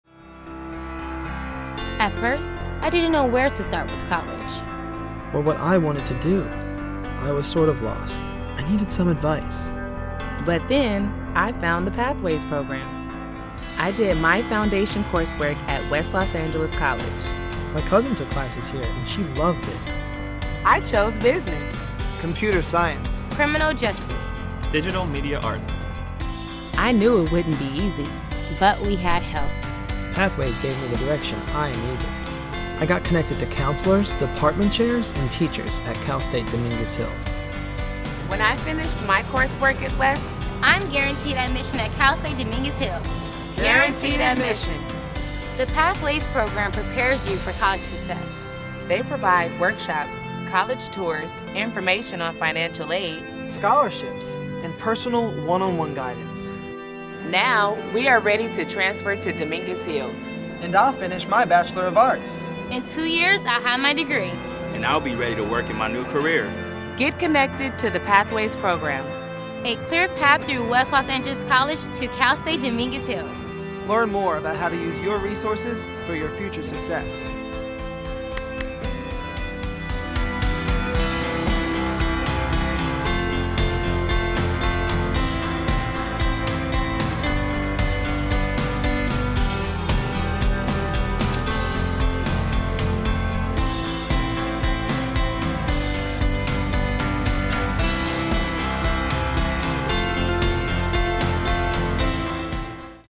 Pathways Academy
The Pathways Academy is a partnership between West Los Angeles College and CSU Dominguez Hills (CSUDH).

Produced by the Digital Design Studio at West Los Angeles College